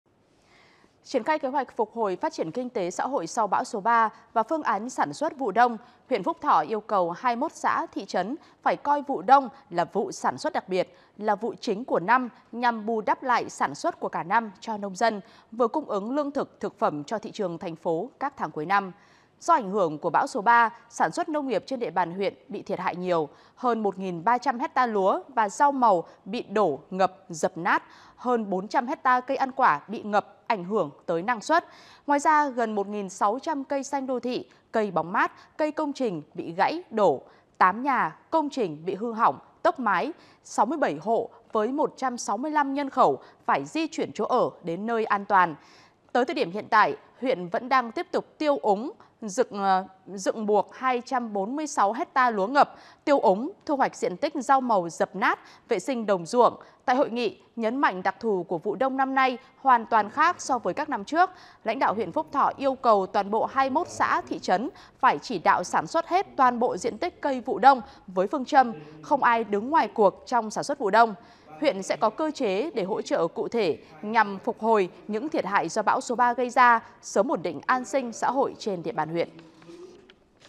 Phúc Thọ tập trung phục hồi phát triển kinh tế sau bão | Tin tức
Triển khai kế hoạch phục hồi phát triển kinh tế, xã hội sau bão số 3 và phương án sản xuất vụ Đông, huyện Phúc Thọ yêu cầu 21 xã, thị trấn phải coi vụ Đông là vụ sản xuất đặc biệt, là vụ chính của năm nhằm bù đắp lại sản xuất của cả năm cho nông dân, vừa cung ứng lương thực, thực phẩm cho thị trường thành phố các tháng cuối năm.